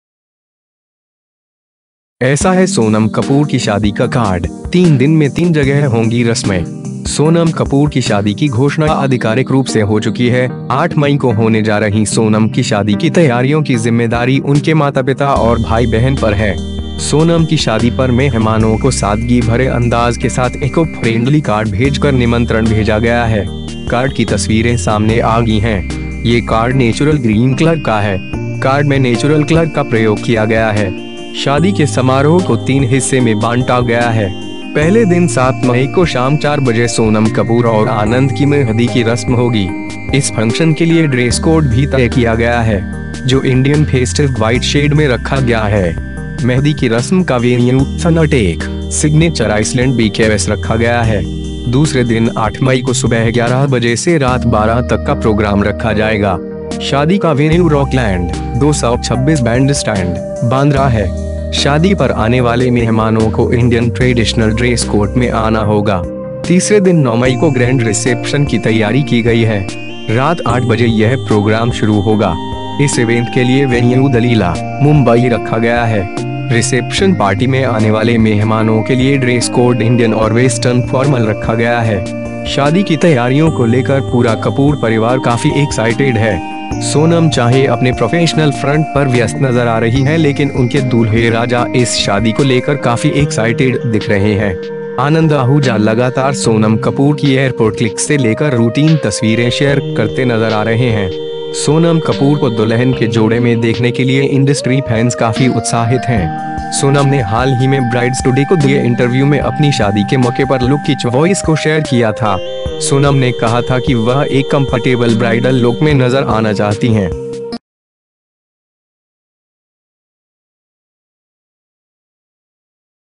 ऐसा है सोनम कपूर की शादी का कार्ड, 3 दिन में 3 जगह होंगी रस्में
सोनम कपूर की शादी की घोषणा आधि‍कारिक रूप से हो चुकी है. 8 मई को होने जा रही सोनम की शादी की तैयार‍ियों की जिम्मेदारी उनके माता-पिता और भाई-बहन पर है.

सोनम की शादी पर मेहमानों को सादगी भरे अंदाज के साथ इकोफ्रेंडली कार्ड भेजकर निमंत्रण भेजा गया है. कार्ड की तस्वीरें सामने आ गईं हैं. ये कार्ड नेचुरल ग्रीन कलर का है.

कार्ड में नेचुरल कलर का प्रयोग किया गया है. शादी के समारोह को तीन हिस्से में बांटा गया है.

पहले दिन 7 मई को शाम 4 बजे सोनम कपूर और आनंद की मेंहदी की रस्म होगी. इस फंक्शन के लिए ड्रेस कोड भी तय किया गया है. जो इंडियन फेस्ट‍िव-वाइट शेड में रखा गयाहै. मेंहदी की रस्म का वेन्यू सनटेक, सिग्नेचर आइसलैंड BKS रखा गया है.

दूसरे दिन 8 मई को सुबह 11 बजे से रात 12 तक का प्रोग्राम रखा जाएगा. शादी का वेन्यू रॉकलैंड, 226 बैंडस्टैंड, बांद्रा है. शादी पर आने वाले मेहमानों को इंडियन ट्रेडिशनल ड्रेस कोड में आना होगा.

तीसरे दिन 9 मई को ग्रैंड रिसेप्शन की तैयारी की गई है. रात 8 बजे यह प्रोग्राम शुरू होगा. इस इवेंट के लिए वेन्यू द लीला, मुंबई रखा गया है. रिसेप्शन पार्टी में आने वाले मेहमानों के लिए ड्रेस कोड इंड‍ियन और वेस्टर्न फॉर्मल रखा गया है.

शादी की तैयारियों को लेकर पूरा कपूर परिवार काफी एक्साइटेड है. सोनम चाहे अपने प्रोफेशनल फ्रंट पर व्यस्त नजर आ रही हैं लेकिन उनके दूल्हे राजा इस शादी को लेकर काफी एक्साइटेड द‍िख रहे हैं. आनंद आहूजा लगातार सोनम कपूर की एयरपोर्ट क्ल‍िक्स से लेकर रूटीन तस्वीरें शेयर करते नजर आ रहे हैं.

सोनम कपूर को दुल्हन के जोड़े में देखने के लिए इंडस्ट्री फैन्स काफी उत्साहित हैं. सोनम ने हाल ही में ब्राइड्स टुडे को दिए इंटरव्यू में अपनी शादी के मौके पर लुक की च्वॉइस को शेयर किया था. सोनम ने कहा था कि वह एक कंफर्टेबल ब्राइडल लुक में नजर आना चाहती हैं.